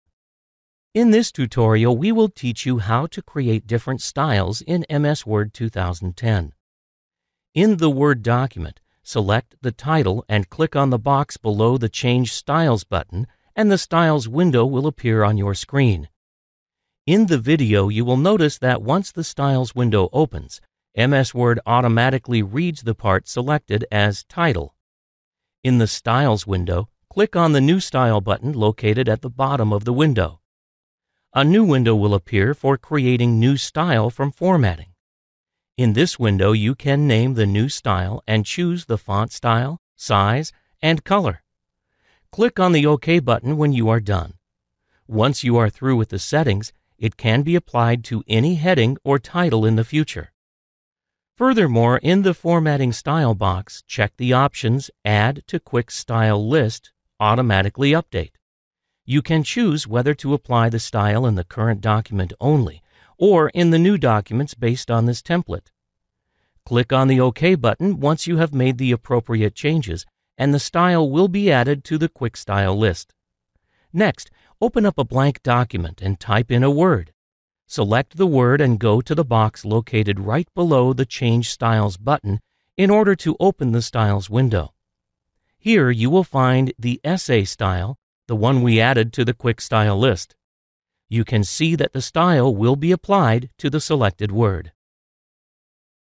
How to Create Styles in Microsoft Word 2010
In this tutorial you will specifically learn the way How to create styles in MS Word 2010.

Presentation plays an important role in capturing a person's interest in a document or report. Word 2010 offers a great collection of tools that can be used to style a document.  It can apply specific styles to the entire document or to a specific part of it. But here the question arises as to whether you only have to use the set of styles offered by the software, or if you can create your own from scratch too. Using this office 2010 tutorial you will learn how to create a personalized style for your word document from scratch.

Step # 1 -- Creating a New Style

In order to create a new style for a document you need to open a word document and select the text you want to create a style for. Let's say, here you want the text in the title area to be styled according to your own preference. In order to do this, you must first click on the 'change Style' dialogue launcher box. Once this appears on your screen, you can see that MS word automatically reads this text as 'Title' because it is located in the title box. In the lowest extreme left corner of the dialogue box you will see a button named 'New Style'. Now you must click on this button.

Step # 2-Creating New Styles from Formatting

Now, a new dialogue box will appear on the front of your screen. This dialogue box will be titled 'creating new styles from formatting'. Once you choose various options and design a style according to your own specifications, this format can be used for any text or titles in future. You must now type the title in the name field, and can make changes to the various formatting parameters i.e. font style, size and color for the particular style.

Step # 3- Adding styles to the Library

Once you have created a new style and want it to use for future documents, you can do so by using the same dialogue. Other options in the formatting dialogue box include, 'Add to Quick Style list', 'automatically update', and 'new documents based on this template' .These options can help you select the same template for future work. You can click on one of these options and select 'OK' to confirm your choice.

Step # 4- Testing the styles created

Once this is done you also need to verify whether all the work you did was fruitful or not. You must now open up a blank document, type a word and select it. You can now go to the 'styles' dialogue launcher box and click on the option 'Essay' in order to select the format you added previously. Now you will be able to see that the new word previously selected will be formatted according to the style you had previously created and applied.